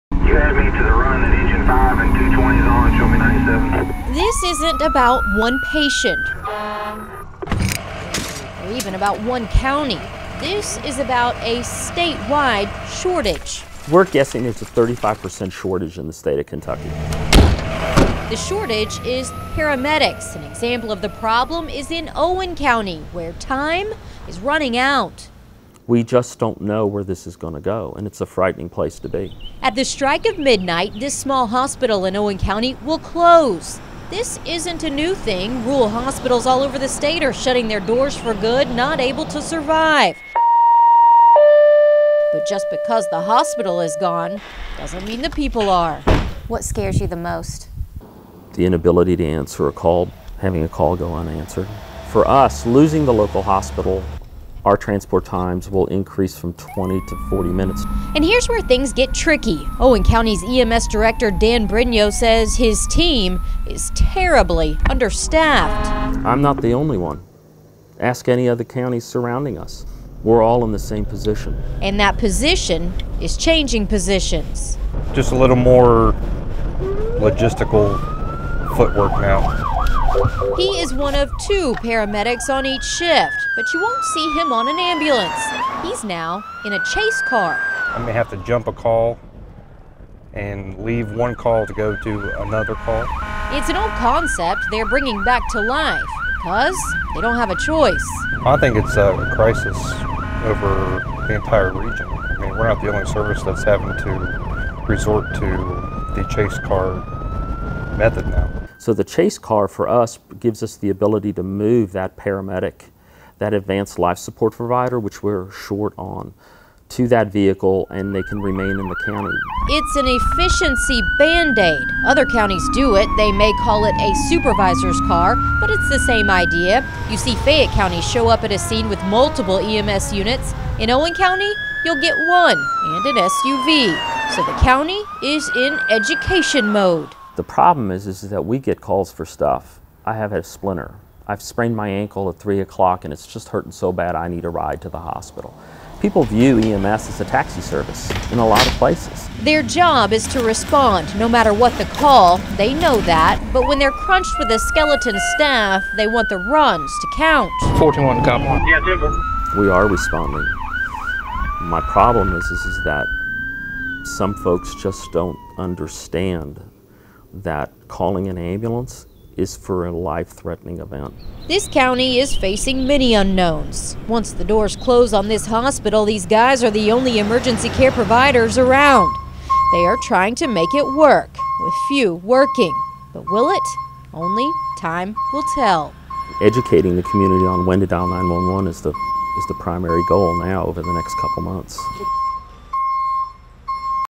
Kentucky's Paramedic Shortage
When you call 911, you expect an ambulance to get to you fast.  But what if one doesn’t show?   That’s close to reality in many Kentucky counties.  It’s so bad in small counties that some have had to go back to the days of the ‘chase car’ where the paramedic  follows the ambulance to the scene.  That way, the paramedic is freed up to go to another emergency, if needed.  In Lewis County, a tiny hospital is shutting down making EMS the only emergency care for miles around.  Their EMS is so short-handed, they say they are having to educate the community about what is, and what is not an emergency.